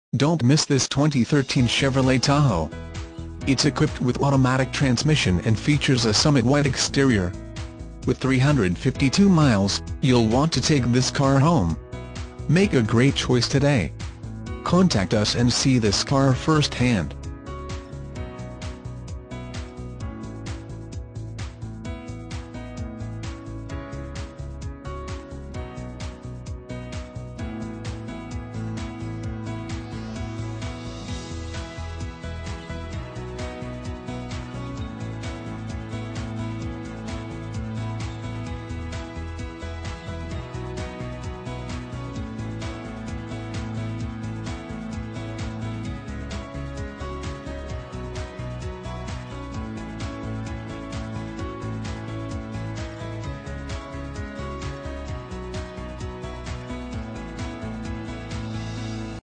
2013 CHEVROLET TAHOE Austin, TX DR105314
2013 CHEVROLET TAHOE Austin, TX
Stock# DR105314

Henna Chevrolet
8805 North IH-35

You'll love this 2013 Chevrolet Tahoe. This is a car you'll want to take home. With 355 miles, it features Automatic transmission and an exterior color of Summit White. Call us and be the first to open the car door today!